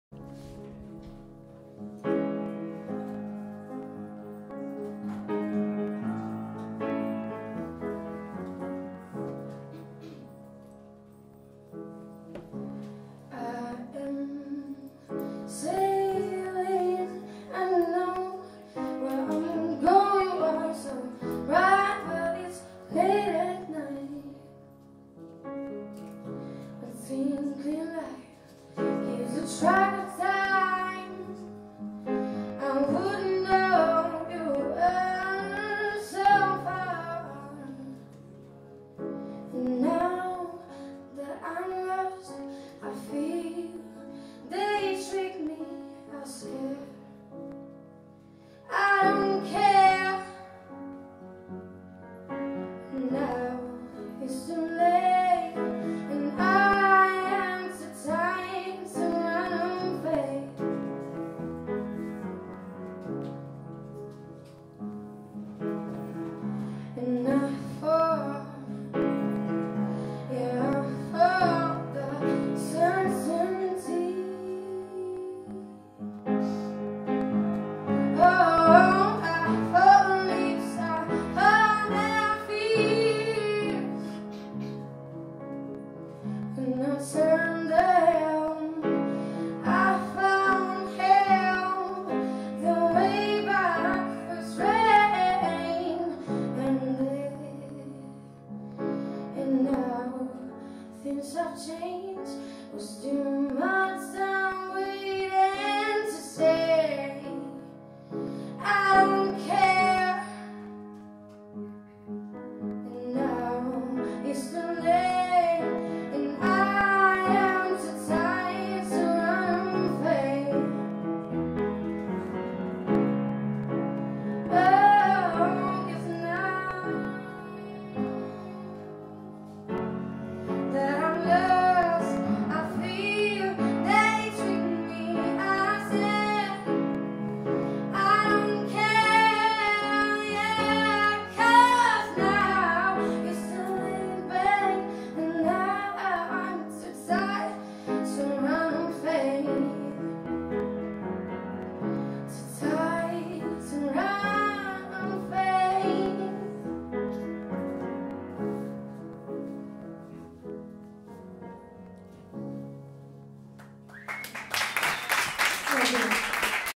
Morgan (Libertad 8, 15/05/13)
Concierto Angela Biedma & Morgan en el Libertad 8 (15/05/13).